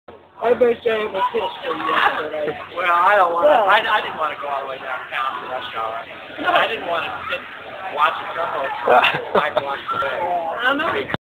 Feeding Duck at Johnny Finn's
I was eating lunch with my parents at Johnny Finn's off of Lake Travis, and a duck was just staring at me the whole time. It was like a dog at a dinner table. So I fed it some French Fries.